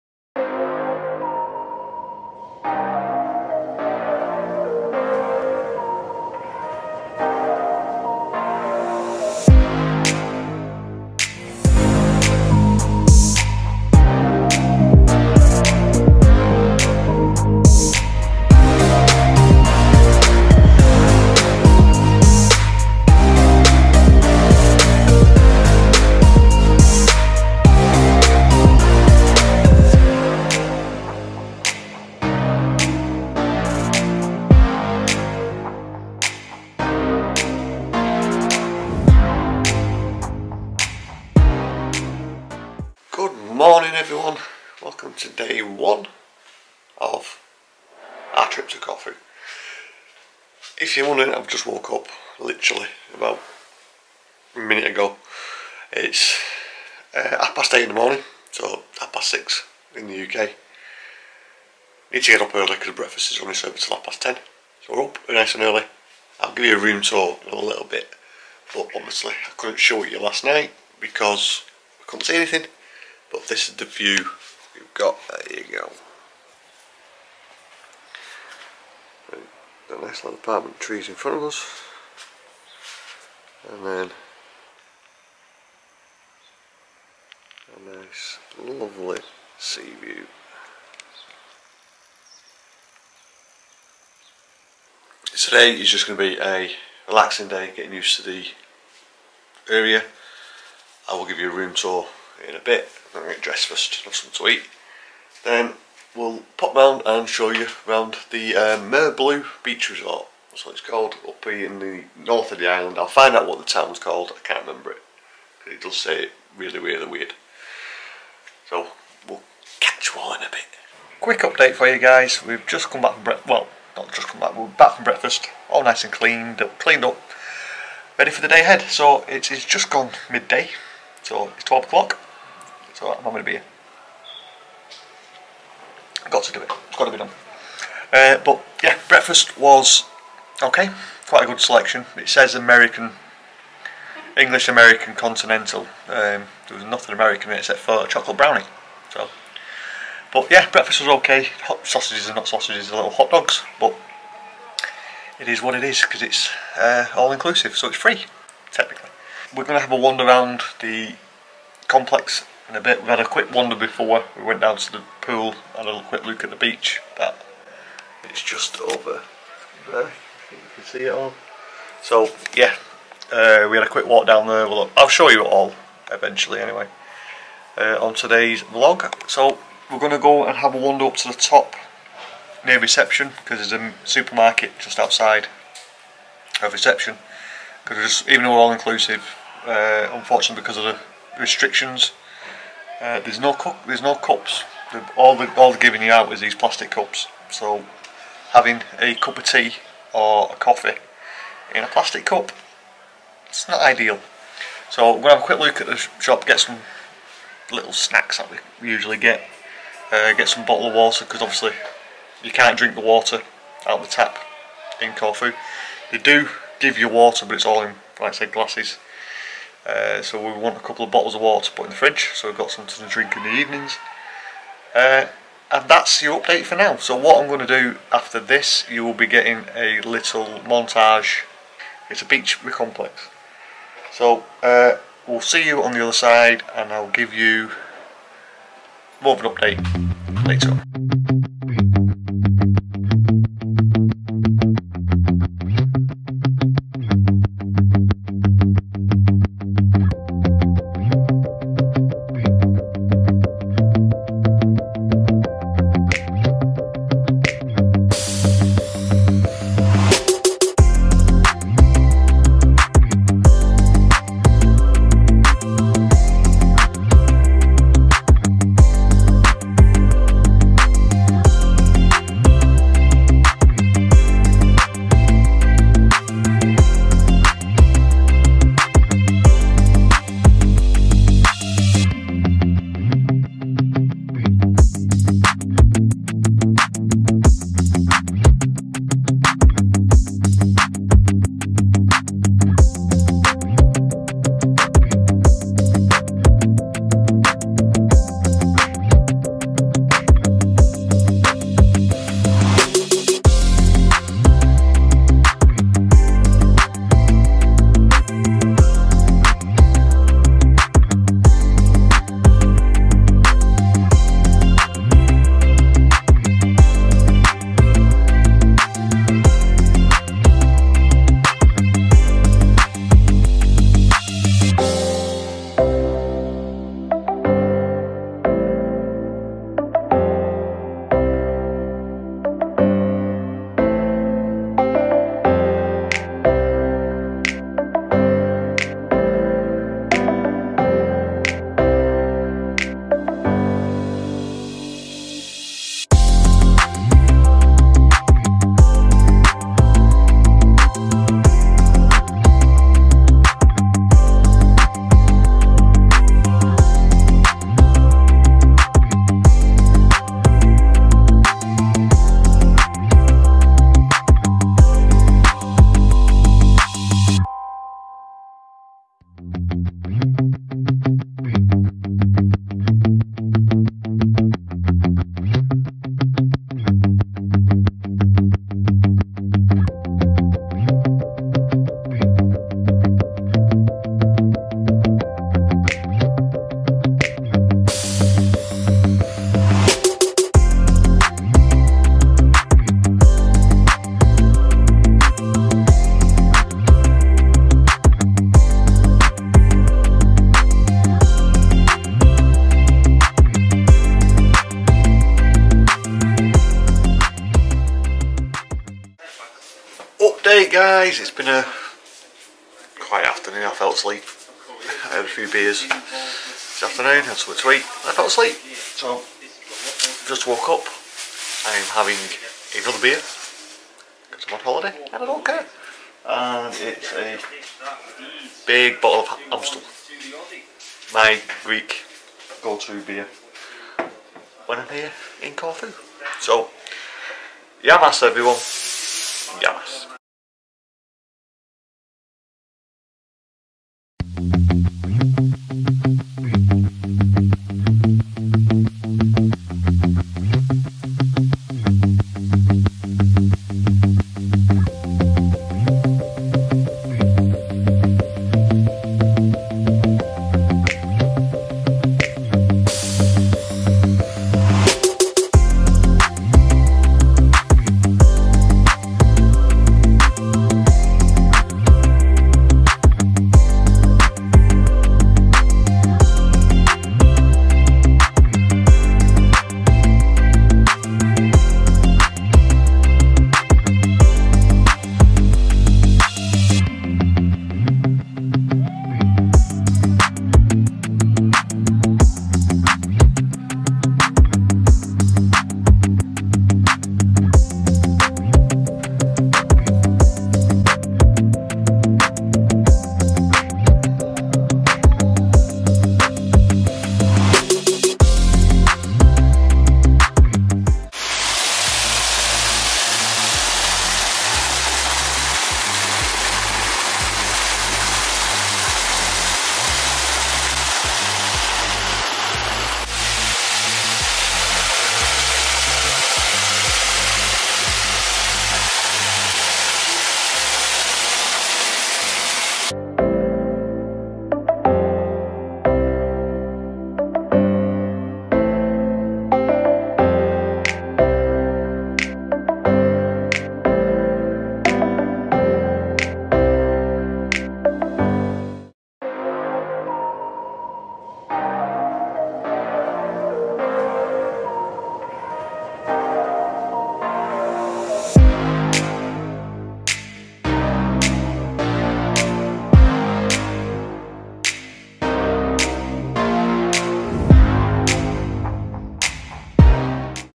Corfu Day 1 at the Mareblue Beach Resort
Hi there,
This is a little series from our recent trip y corfu in sept 2020,
join us at we travel to the wonderful island of corfu and enjoy the local beer and foods.